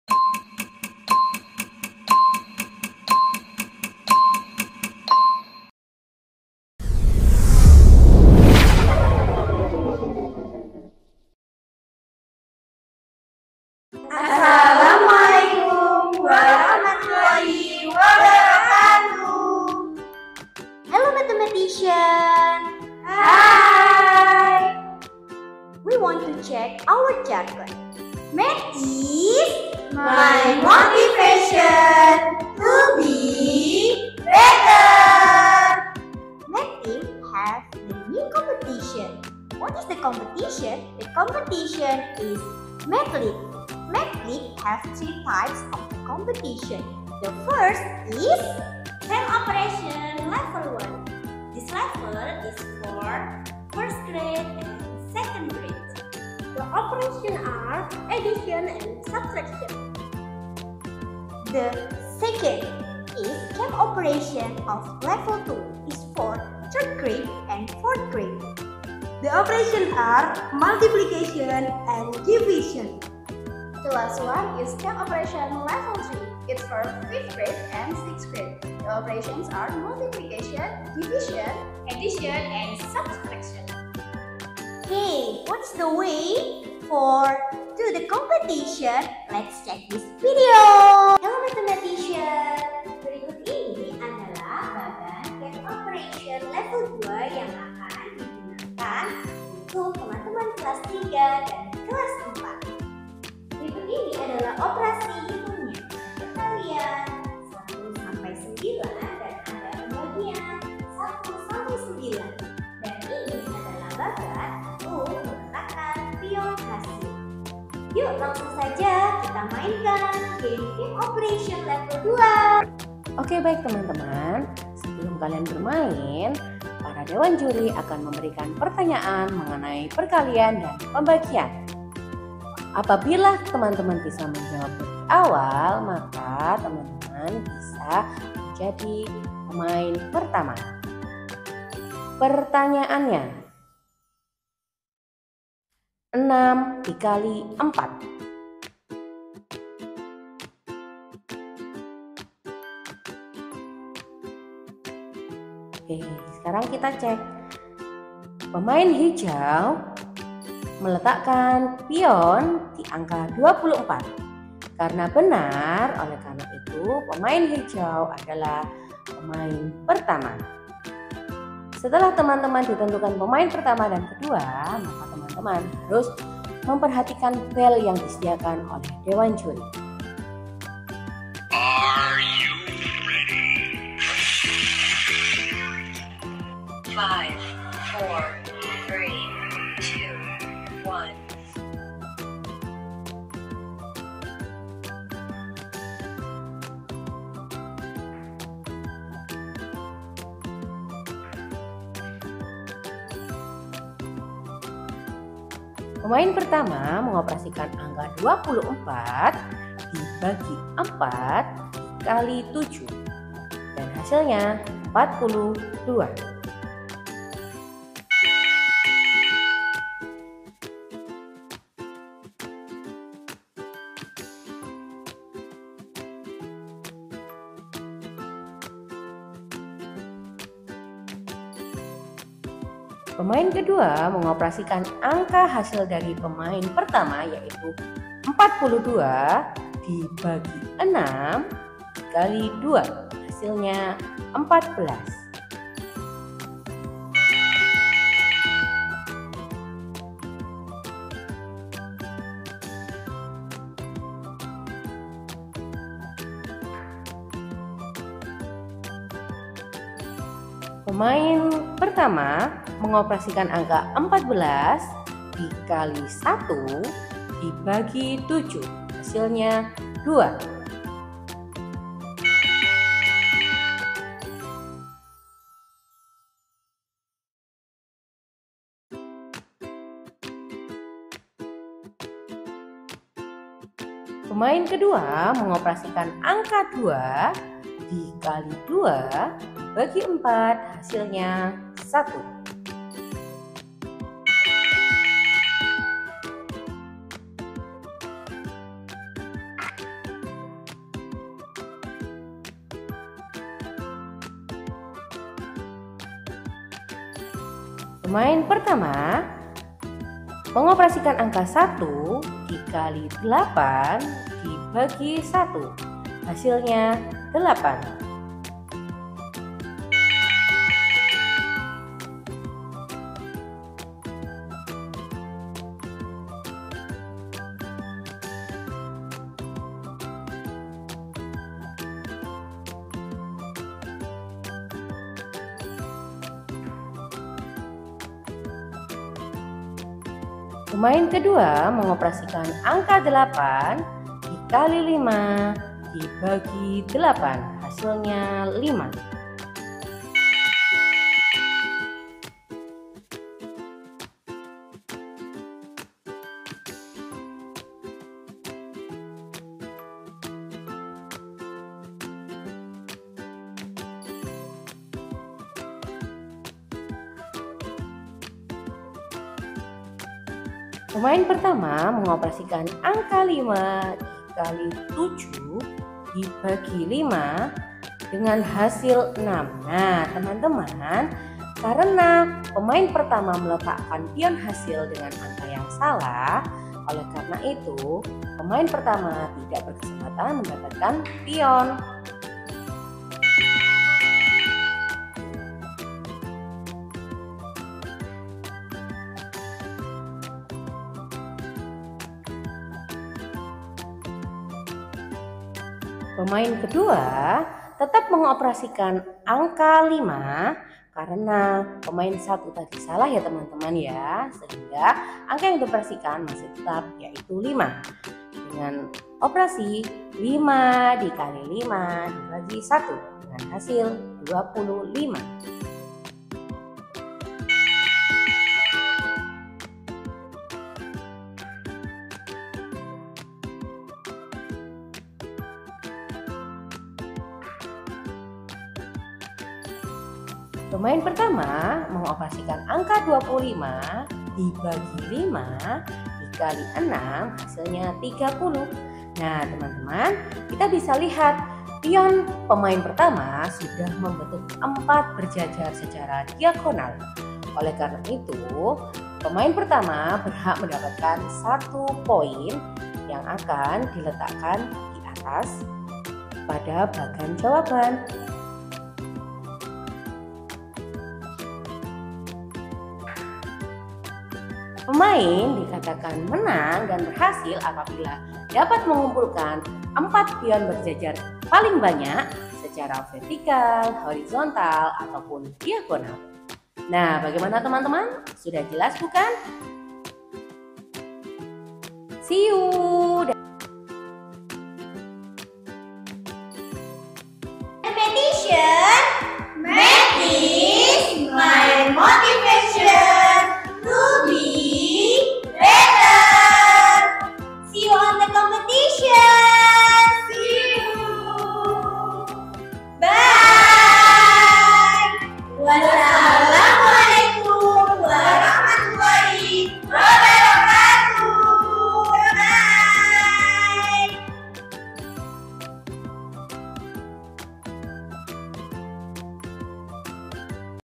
TUTORIAL CAMP OPERATION LEVEL 2 (MATHEMATICS LEAGUE)
Hello guys.. this is tutorial of Mathematics League for Level 2.
Let's practice and be the winner!